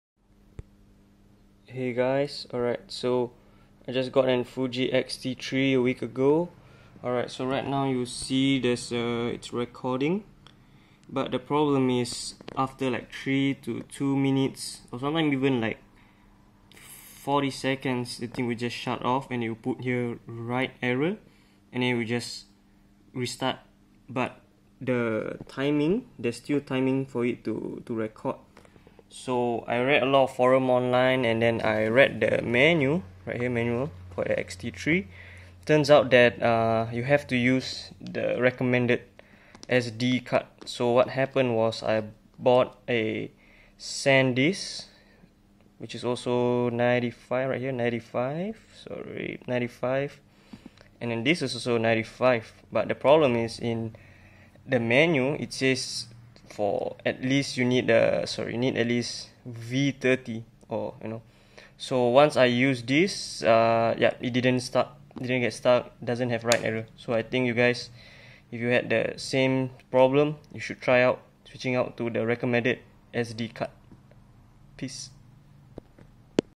Fuji XT3 write error problem
just bought a fuji xt3 but after when I tried to record video in UHD or 4K after like 50 seconds as you can see in the video it stops (55seconds) and says write error.

I read on some forums some people tried to change some settings and also managed to resolve it. but this works for me